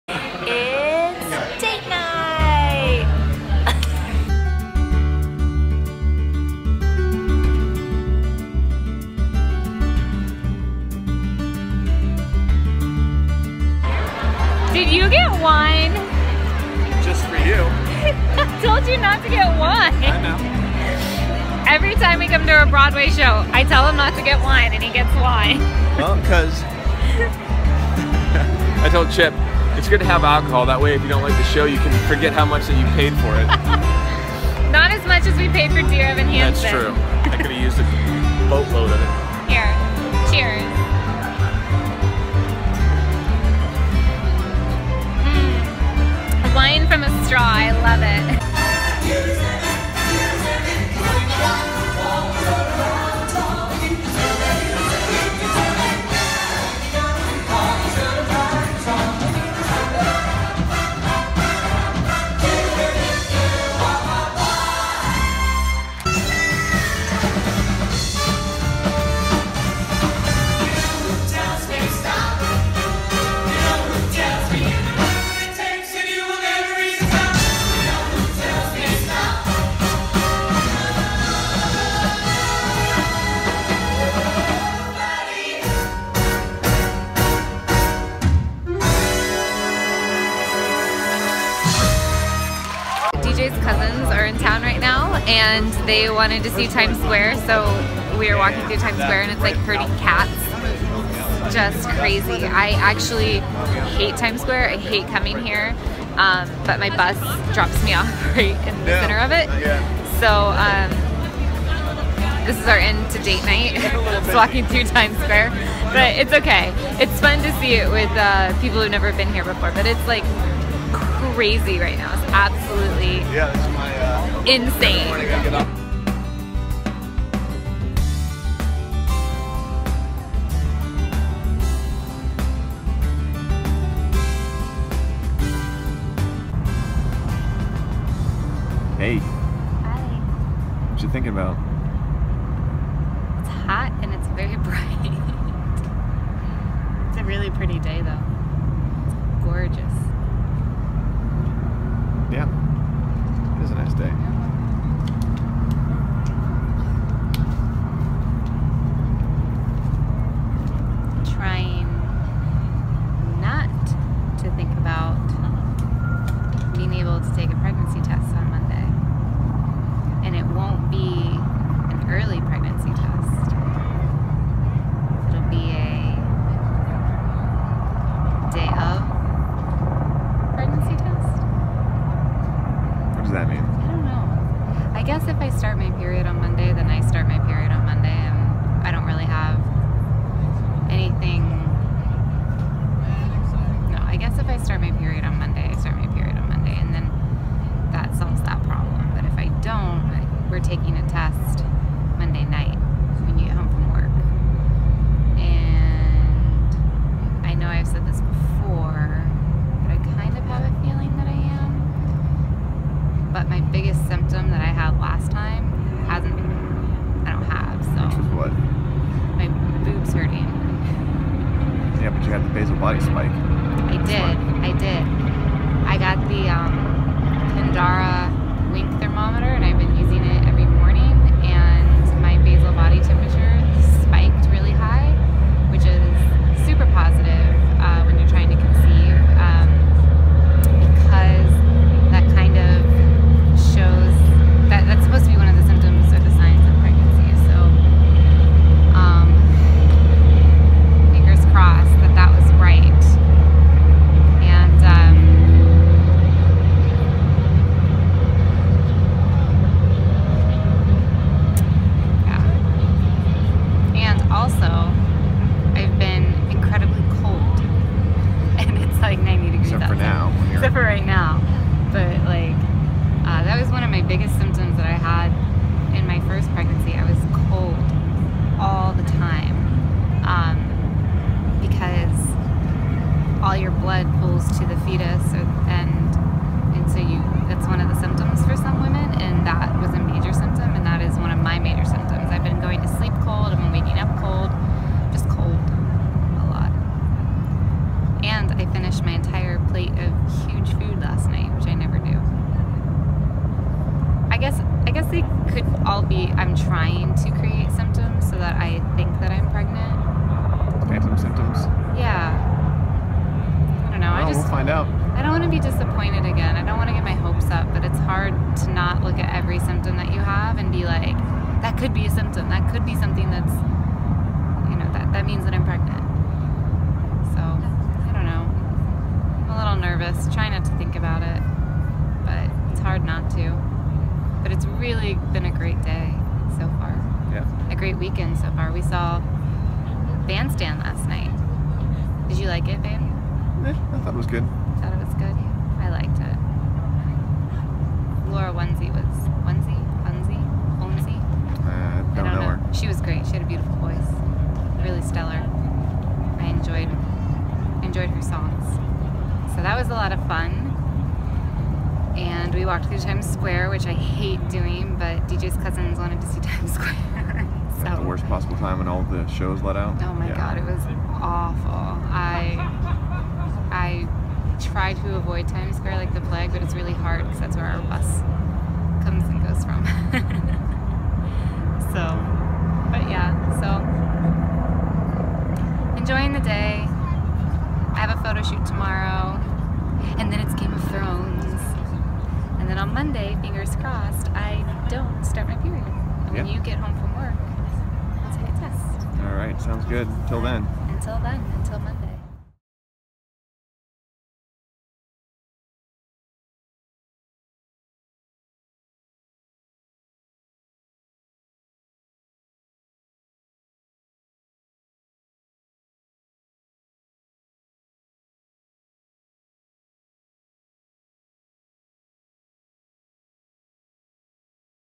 Date Weekend Before Pregnancy Test in New York
My husband planned a great weekend to get my mind off of the end of my cycle and the ever stressful waiting to NOT get my period.

Moving to New York and trying to conceive after our loss in February has really been stressful to me. I try and be positive every month but it's so hard to get a big fat negative on your pregnancy test. So, no more early pregnancy tests. I'm waiting until I'm due, and if it doesn't come I'll take one. We had a great time when the Hubs cousins came into town and we saw Bandstand, the Musical, which was wonderful and then toured Brooklyn. The waiting to take a pregnancy test is always hard, but feeling symptoms and then getting your period is always so sad. Fingers crossed this time will be worth the wait.